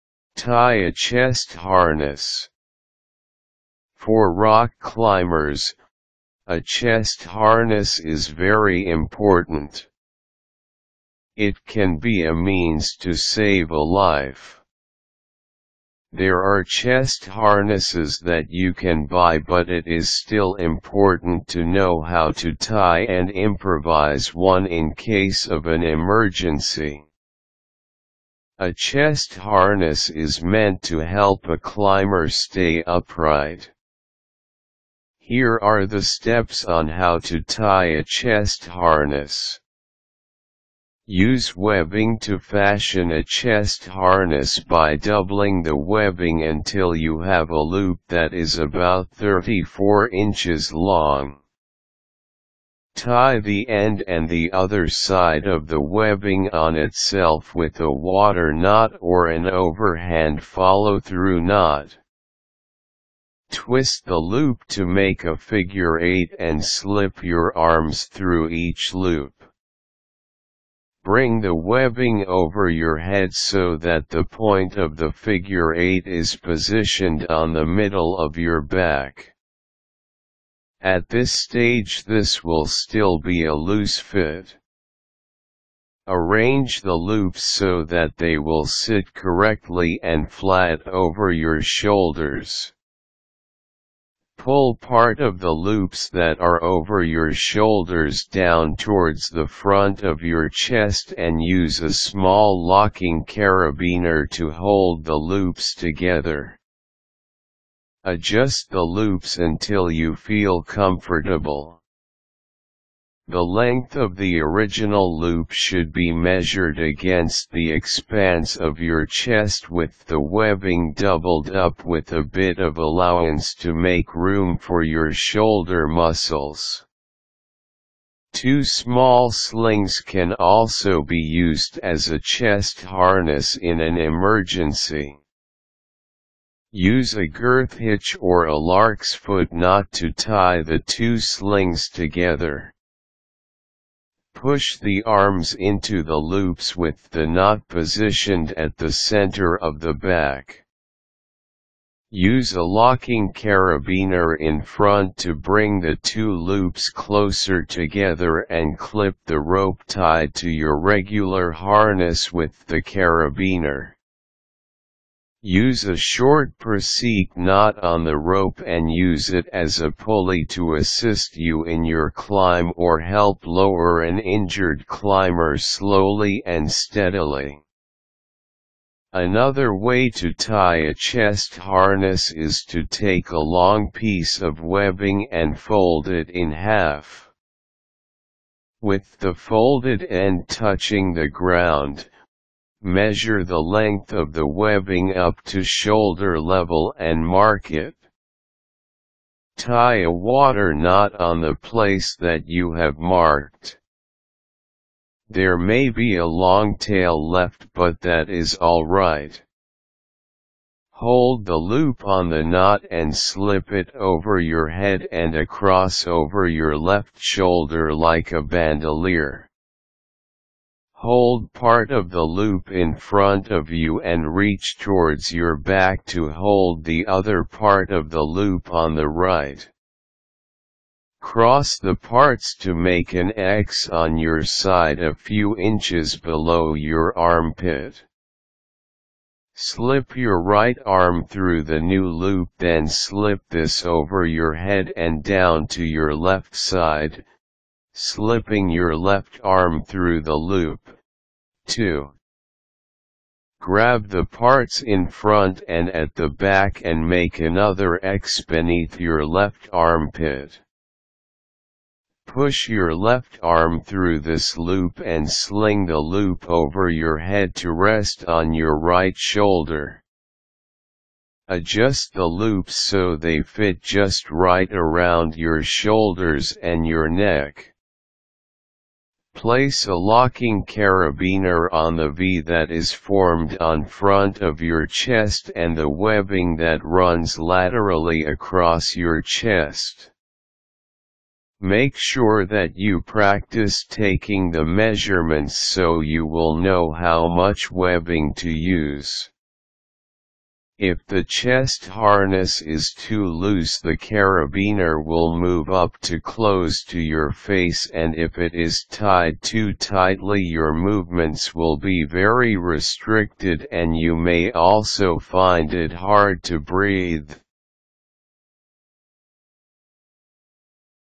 How to : Tie a chest harness
Tie a chest harness

 For rock climbers, a chest harness is very important. It can be a means to save a life. There are chest harnesses that you can buy but it is still important to know how to tie and improvise one in case of an emergency. A chest harness is meant to help a climber stay upright. Here are the steps on how to tie a chest harness.

 Use webbing to fashion a chest harness by doubling the webbing until you have a loop that is about thirty-four inches long. Tie the end and the other side of the webbing on itself with a water knot or an overhand follow-through knot. Twist the loop to make a figure eight and slip your arms through each loop. Bring the webbing over your head so that the point of the figure eight is positioned on the middle of your back. At this stage this will still be a loose fit. Arrange the loops so that they will sit correctly and flat over your shoulders. Pull part of the loops that are over your shoulders down towards the front of your chest and use a small locking carabiner to hold the loops together. Adjust the loops until you feel comfortable. The length of the original loop should be measured against the expanse of your chest with the webbing doubled up with a bit of allowance to make room for your shoulder muscles. Two small slings can also be used as a chest harness in an emergency. Use a girth hitch or a lark’s foot knot to tie the two slings together. Push the arms into the loops with the knot positioned at the center of the back. Use a locking carabiner in front to bring the two loops closer together and clip the rope tied to your regular harness with the carabiner. Use a short prusik knot on the rope and use it as a pulley to assist you in your climb or help lower an injured climber slowly and steadily. Another way to tie a chest harness is to take a long piece of webbing and fold it in half. With the folded end touching the ground, measure the length of the webbing up to shoulder level and mark it. Tie a water knot on the place that you have marked. There may be a long tail left but that is all right. Hold the loop on the knot and slip it over your head and across over your left shoulder like a bandolier. Hold part of the loop in front of you and reach towards your back to hold the other part of the loop on the right. Cross the parts to make an X on your side a few inches below your armpit. Slip your right arm through the new loop then slip this over your head and down to your left side, slipping your left arm through the loop, too. Grab the parts in front and at the back and make another X beneath your left armpit. Push your left arm through this loop and sling the loop over your head to rest on your right shoulder. Adjust the loops so they fit just right around your shoulders and your neck. Place a locking carabiner on the V that is formed on front of your chest and the webbing that runs laterally across your chest.

 Make sure that you practice taking the measurements so you will know how much webbing to use. If the chest harness is too loose the carabiner will move up to close to your face and if it is tied too tightly your movements will be very restricted and you may also find it hard to breathe.